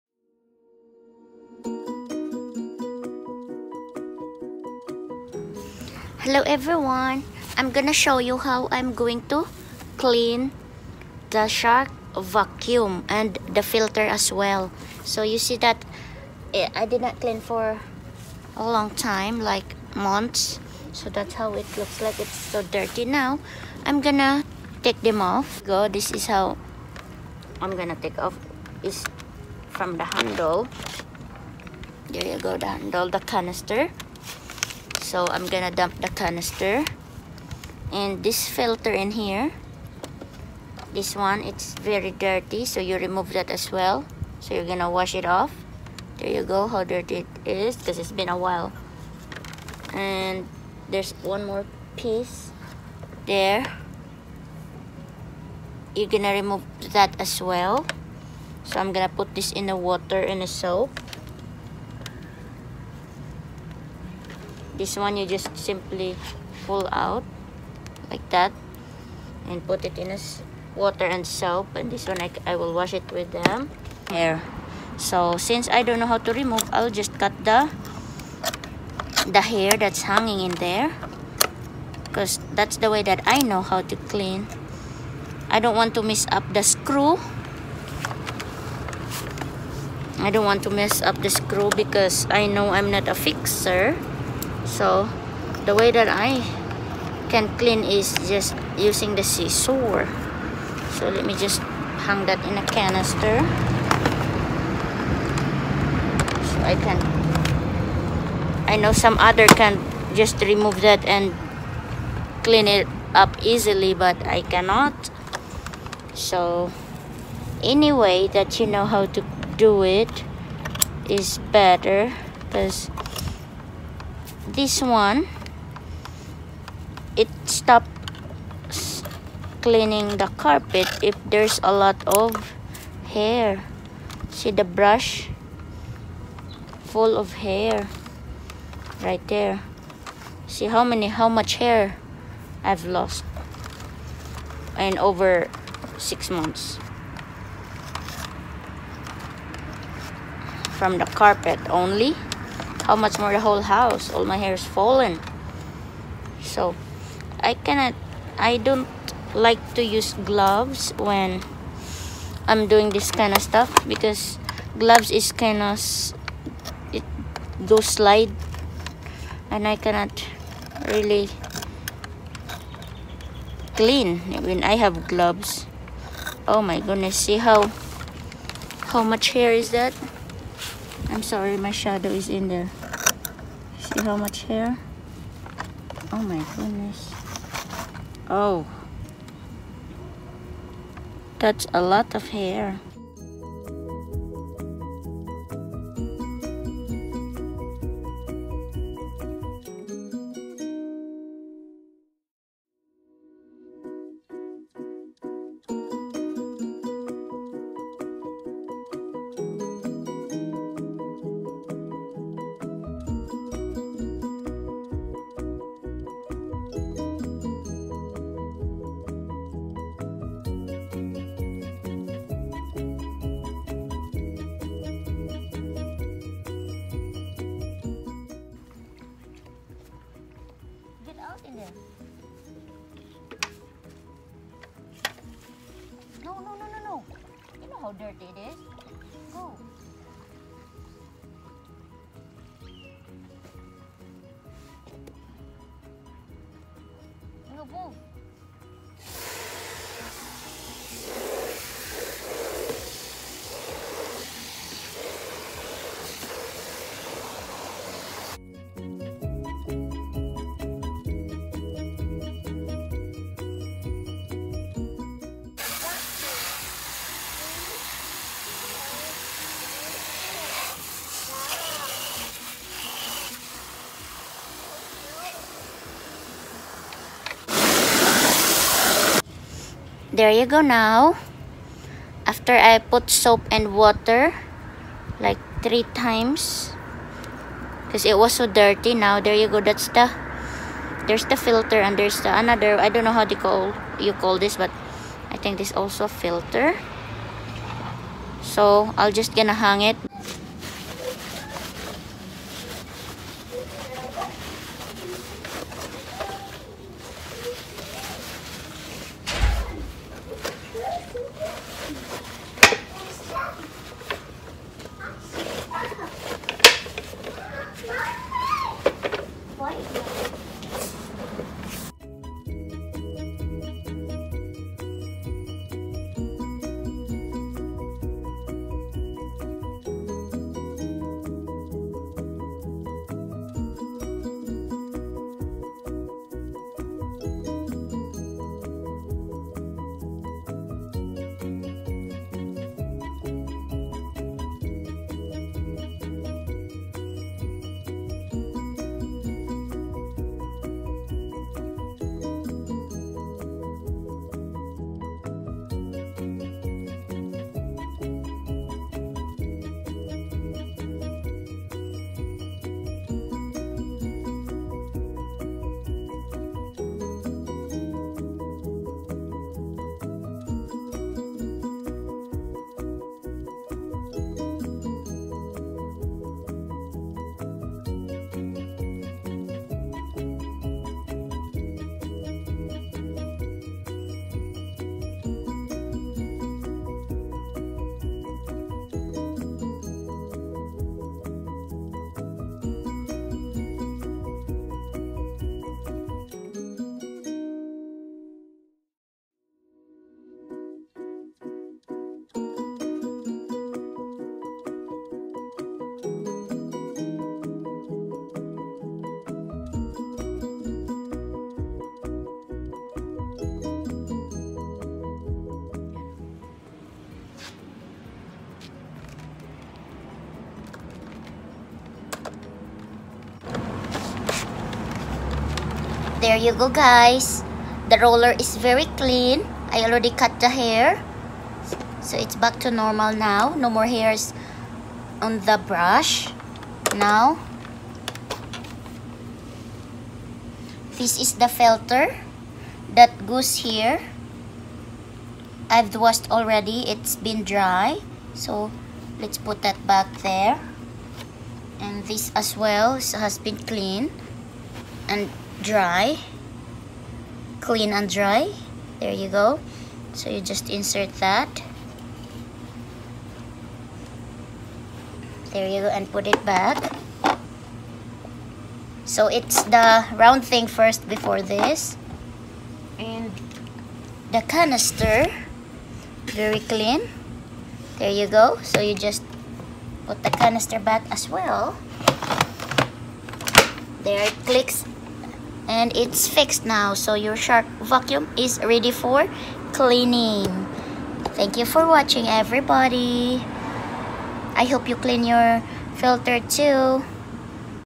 HOW TO CLEAN SHARK VACUUM FILTER [4K ULTRA HD]
Hello guys, this video I am sharing to you how I take care my home vacuum, shark professional, lift-away & canister, enhanced swivel steering, anti-allergen, complete seal.
The brush, the filter and canister cleaned up in my style.